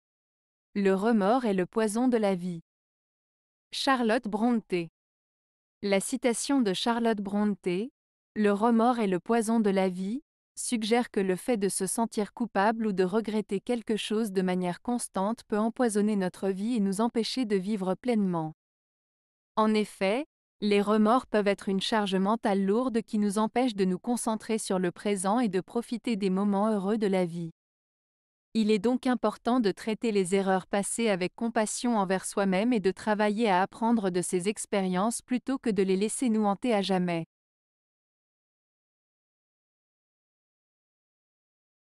Le remords est le poison de la vie. - Charlotte Brontë
La citation de Charlotte Brontë, "Le remords est le poison de la vie", suggère que le fait de se sentir coupable ou de regretter quelque chose de manière constante peut empoisonner notre vie et nous empêcher de vivre pleinement. En effet, les remords peuvent être une charge mentale lourde qui nous empêche de nous concentrer sur le présent et de profiter des moments heureux de la vie. Il est donc important de traiter les erreurs passées avec compassion envers soi-même et de travailler à apprendre de ces expériences plutôt que de les laisser nous hanter à jamais.